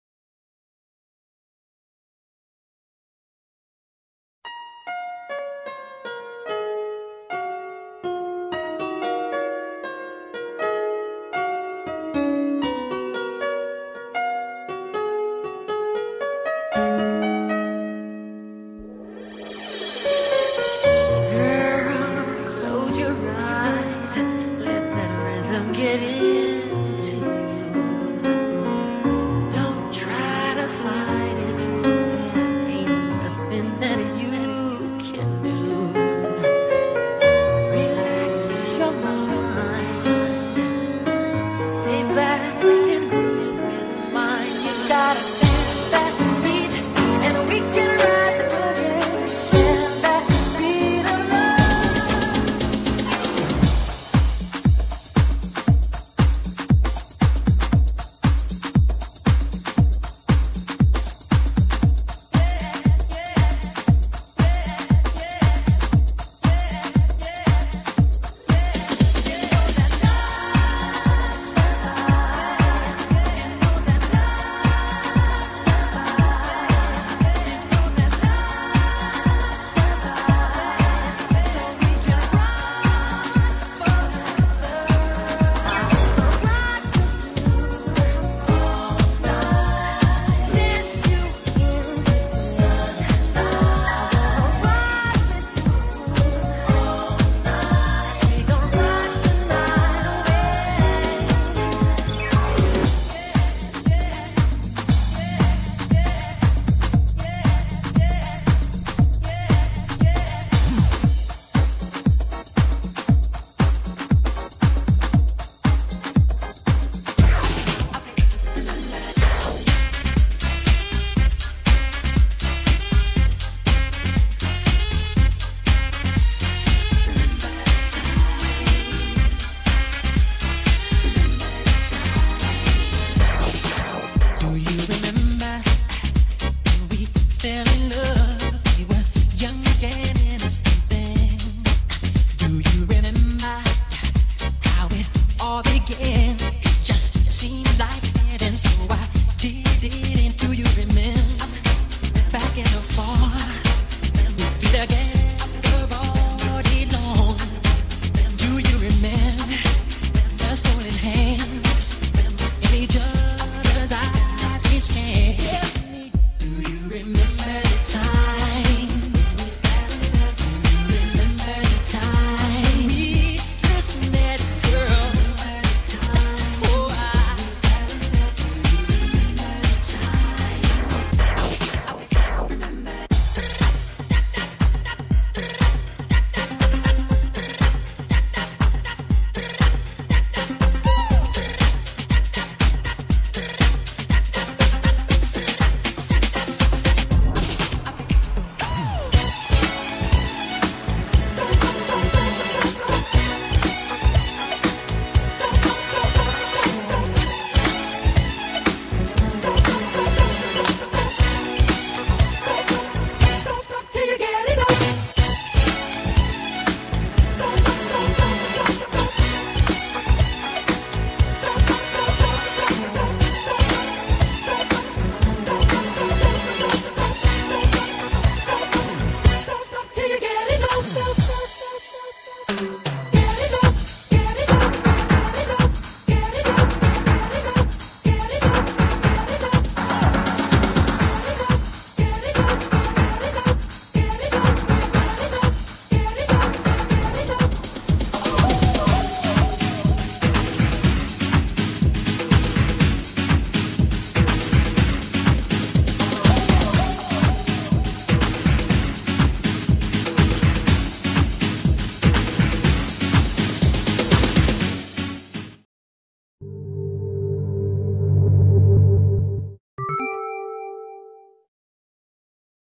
Michael jackson HOT MIX!!!!
this is just a few of my background pic's I used and some random pictures of Michael, enjoy!!!